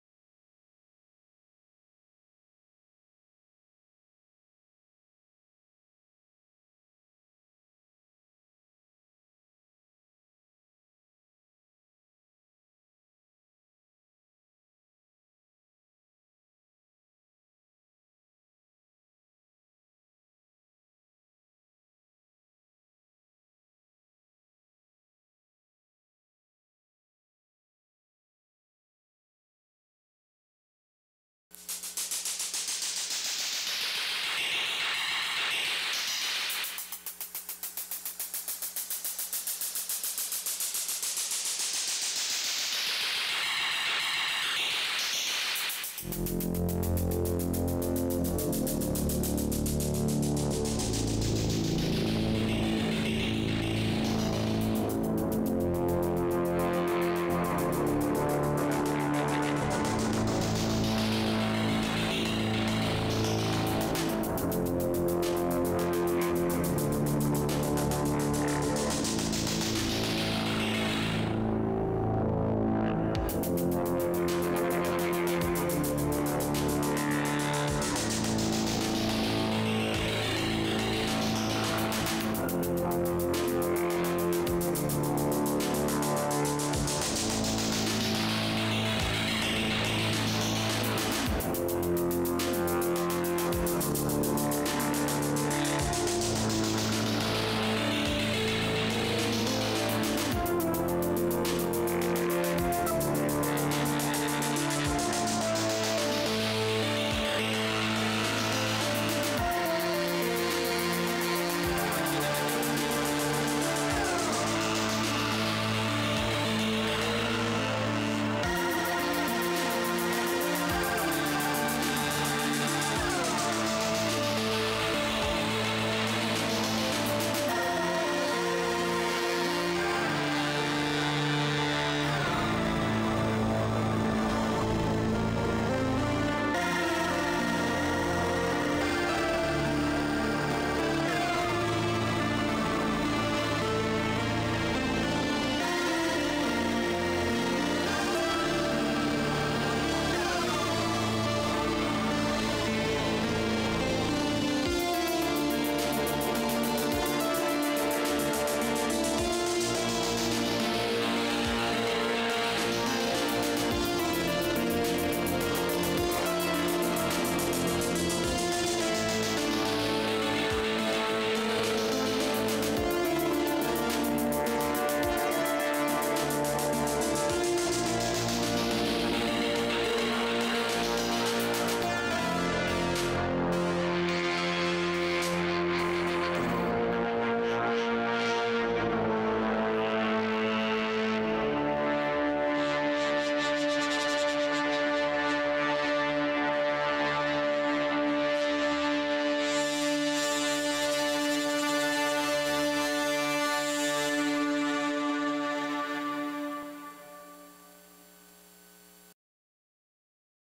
Novation sequencing jam
Novation synth jam with novation circuit sequencing a Novation Summit and Novation Bass Station 2